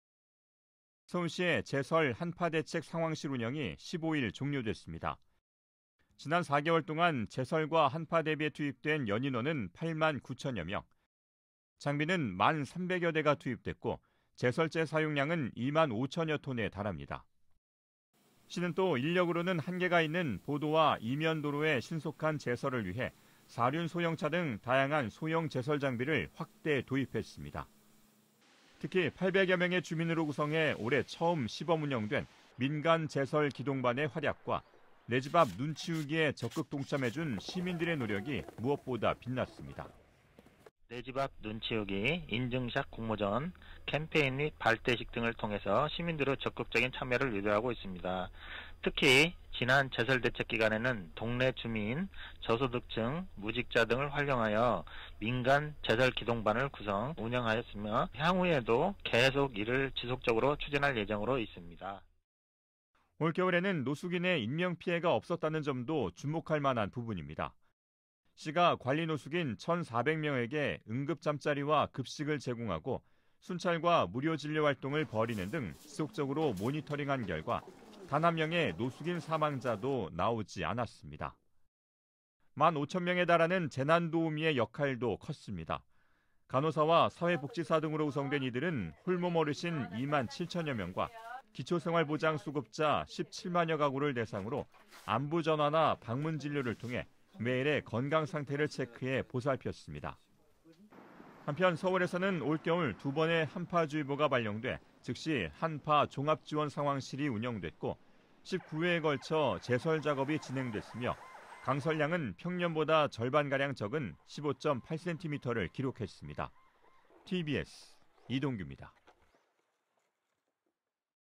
[tbsTV]제설․한파상황실 운영 종료…노숙인 사망자 ‘제로’
지난겨울 24시간 내내 시민들의 안전을 위해 가동됐던 제설․한파대책 상황실이 15일, 운영을 종료했습니다. 올 겨울에는 특히 노숙인 사망자가 단 한 명도 나오지 않았다는데 큰 의미가 있는데요. 서울시는 올해 처음 시범운영했던 민간 제설기동반은 앞으로도 계속 추진하기로 했습니다.